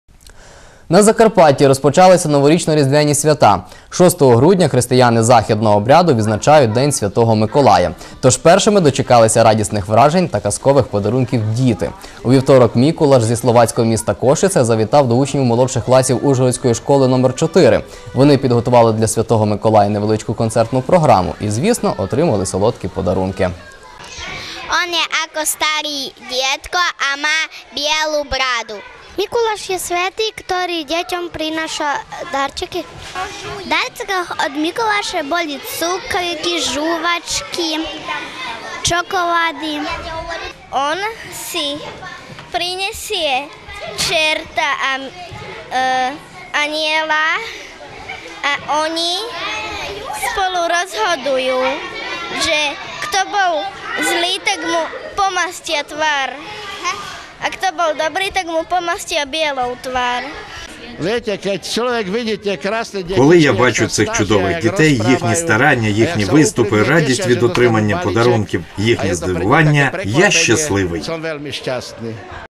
На Закарпатті розпочалися новорічно-різдвяні свята
На Закарпатті розпочалися новорічно-різдвяні свята. 6 грудня християни  західного обряду відзначають день Святого Миколая, тож першими дочекалися радісних вражень та казкових подарунків діти. У вівторок Мікулаш зі словацького міста Кошице завітав до учнів молодших класів ужгородської школи номер чотири. Вони підготували для Святого Миколая невеличку концертну програму, і, звісно, отримали солодкі подарунки.